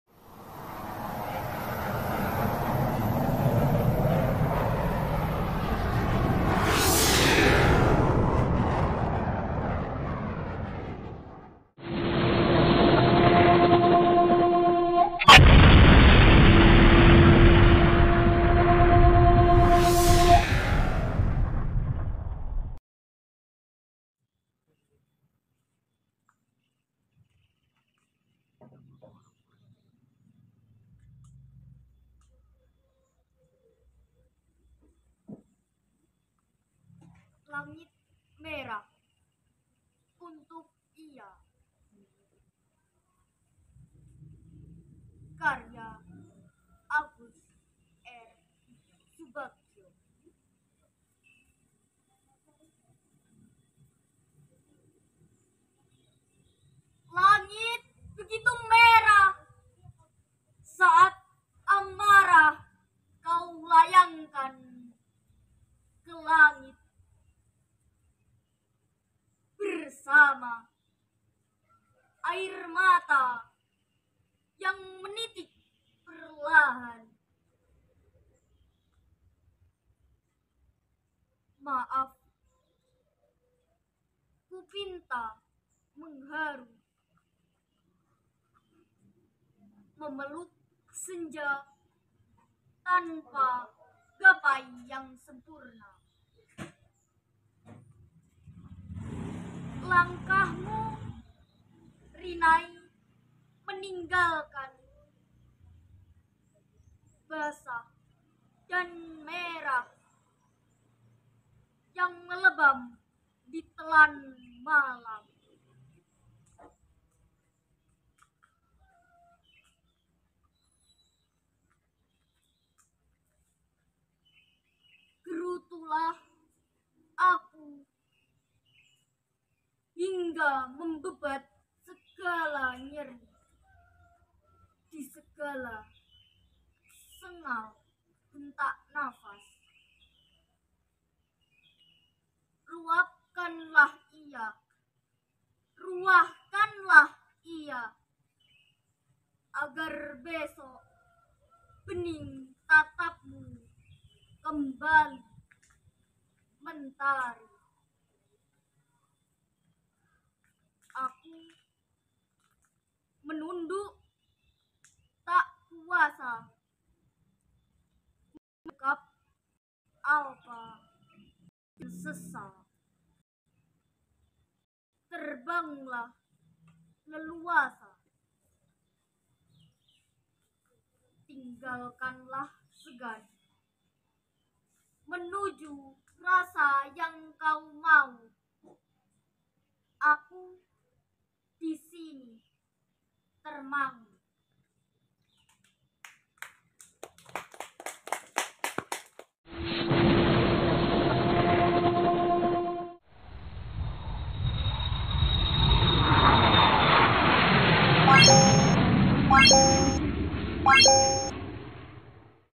Baca Puisi || Langit Merah - Agus R Subagyo || Maora Vietranao Gitaralitha, 13-06-2023
Maora Vietranao Gitaralitha
Kelas 6
MIN 3 Nganjuk
Komunitas Tjaraka
Sanggar Rumah Ilalang
Santriwati @PPQUEENALFALAH

Baca Puisi
Langit Merah
karya Agus R Subagyo

Doc Latihan di MIN 3 Nganjuk: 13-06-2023